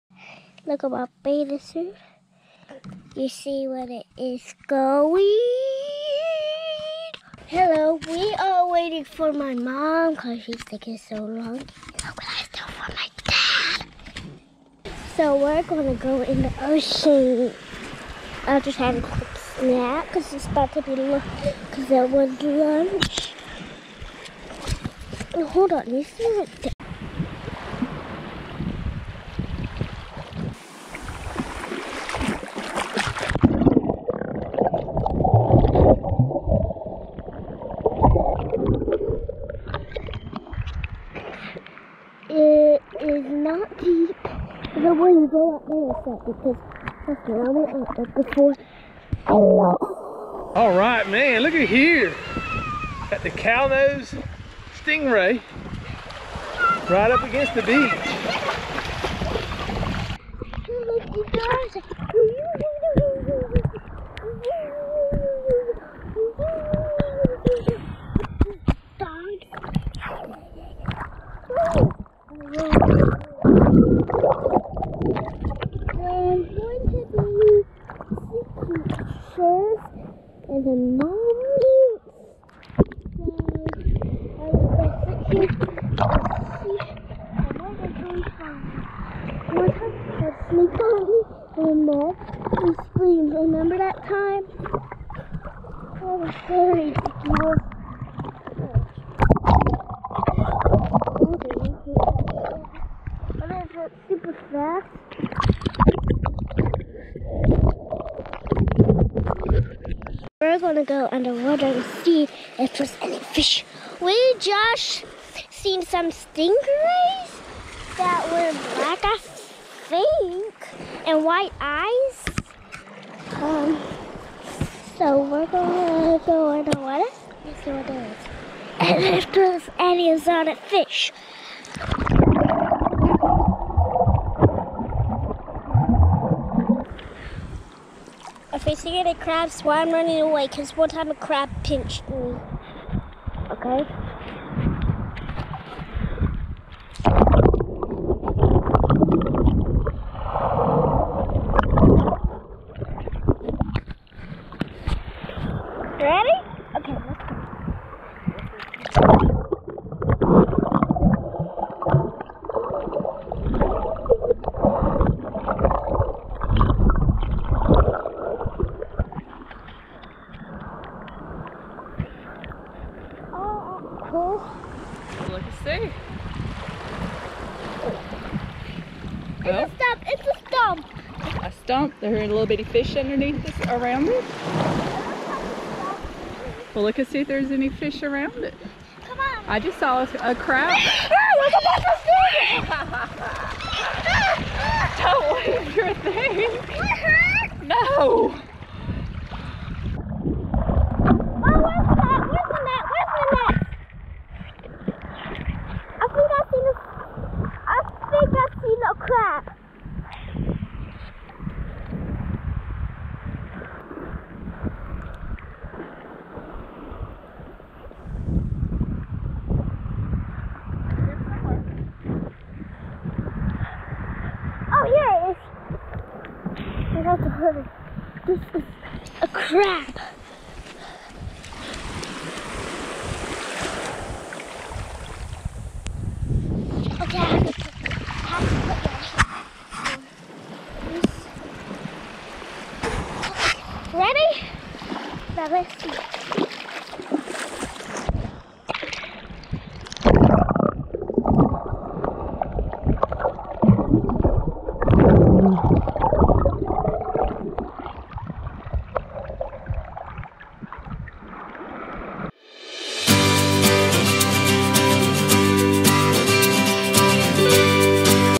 Reece goes to the beach, and swims with Sting Rays! Ep. 4
Reece goes to the beach and is surprised by Sting Rays!